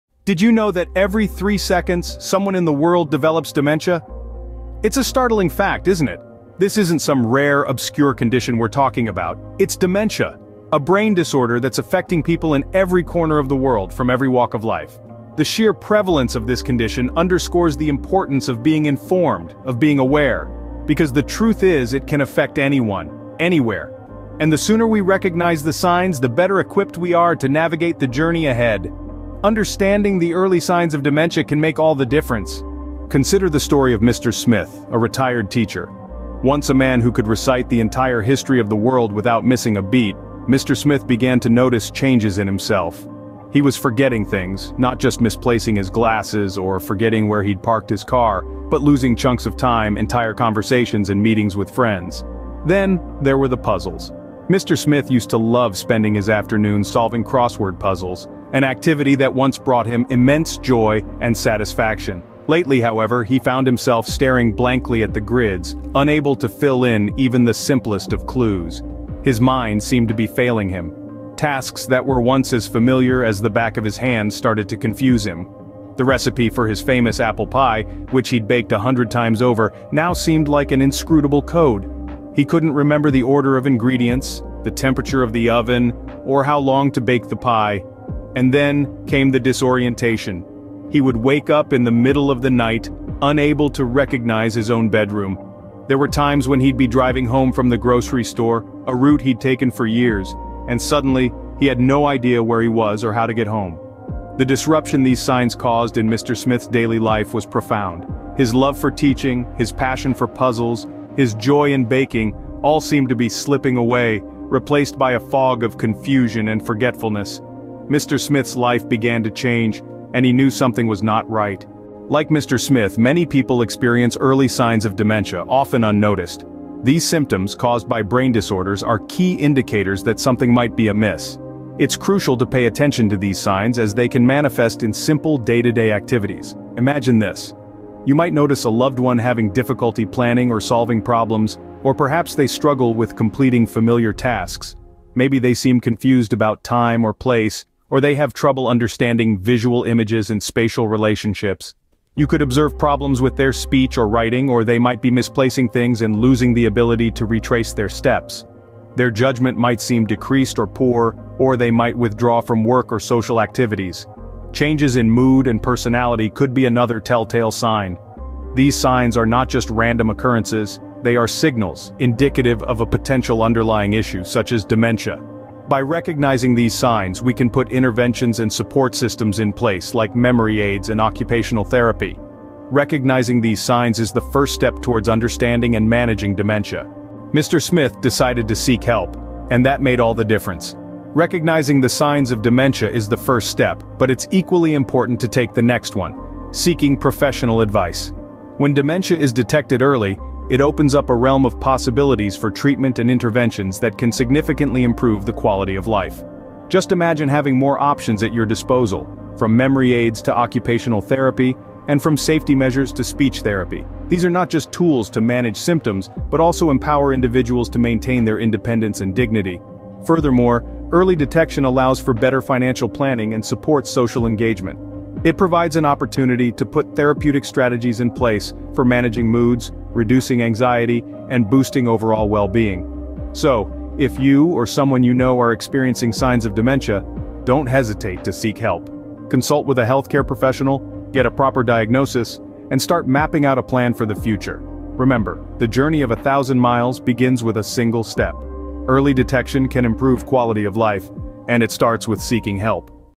Decoding Dementia: Spotting the Early Signs
In this insightful video, we delve into the early signs of dementia, highlighting the criticality of early detection and intervention. Dementia is a condition that affects millions worldwide, and it's crucial to recognize its early indicators to provide timely support and care. We explore common symptoms, such as memory loss, confusion, and behavioral changes, that can be early warning signs of dementia. Learn about the importance of seeking medical advice and taking proactive steps to manage the condition effectively. Join us on this educational journey and help spread awareness by sharing this video with your loved ones. Together, let's make a difference in the lives of those impacted by dementia.

OUTLINE:
00:00:00 - A Startling Statistic
00:00:32 - The Story of Mr. Smith
00:02:01 - Recognizing the Signs
00:03:07 - Seeking Help